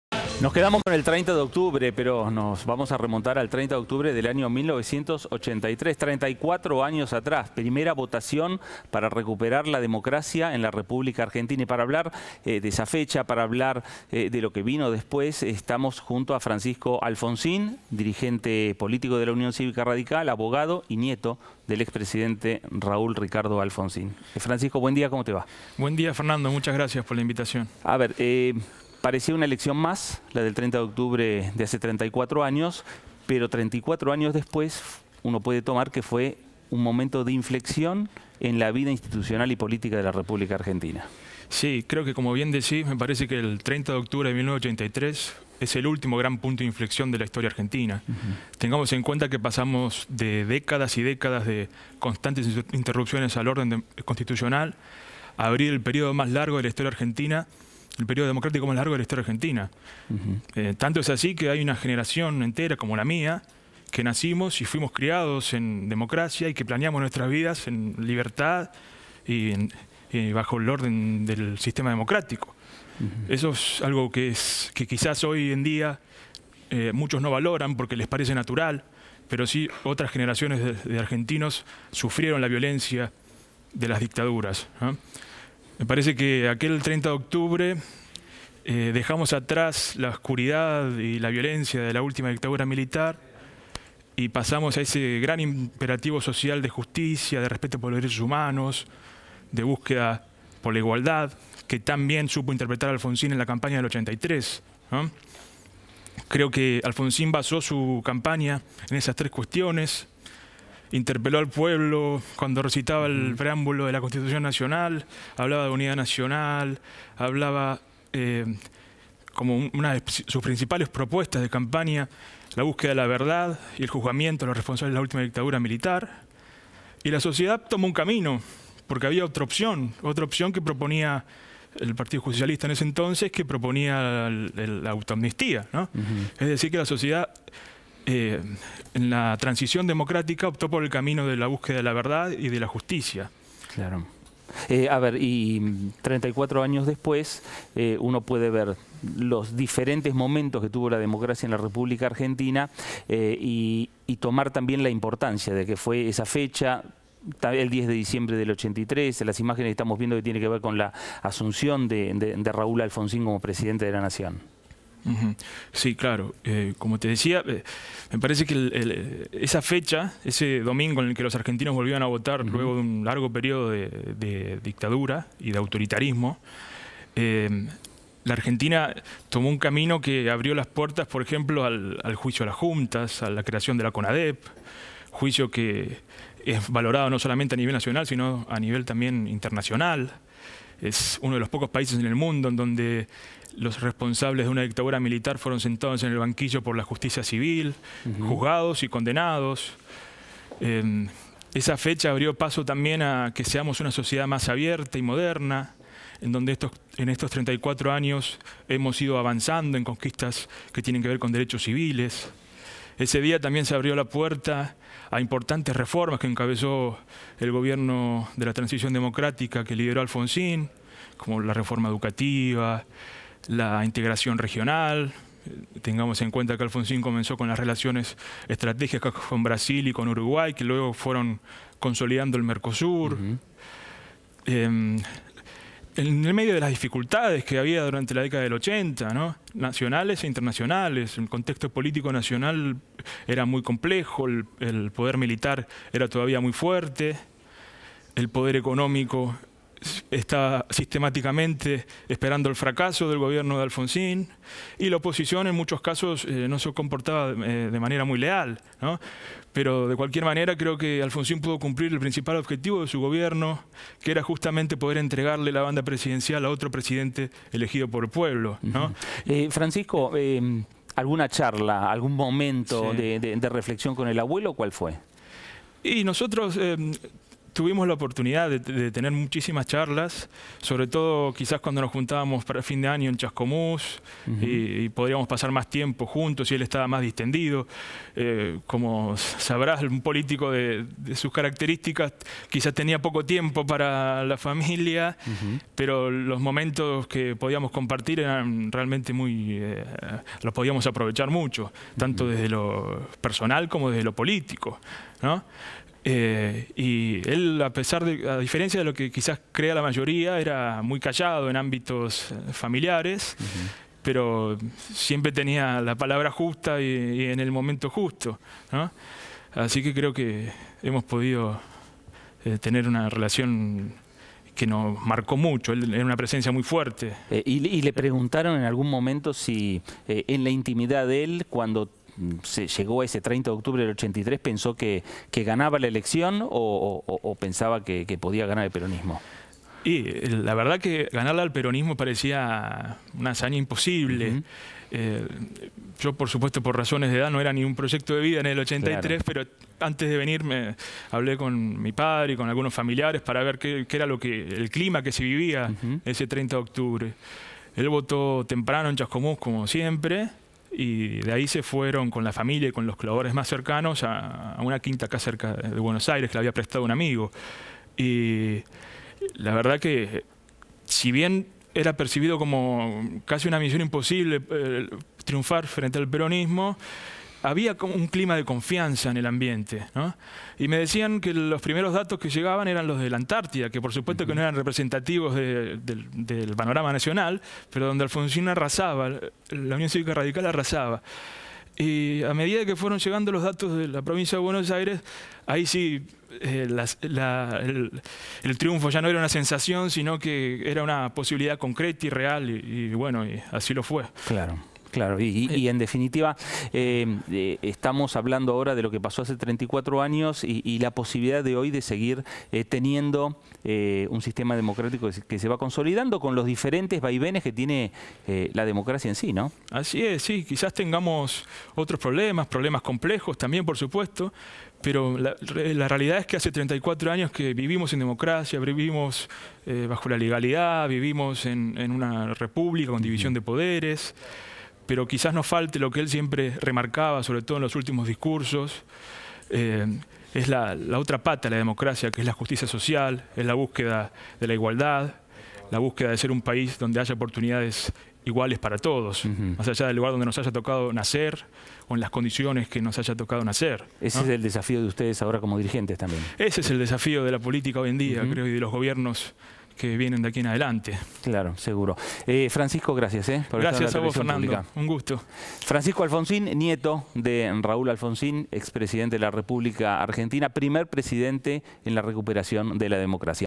Francisco Alfonsín, nieto de Raúl: La democracia argentina
Emitido por TV Pública Argentina, el lunes 30 de octubre de 2017. y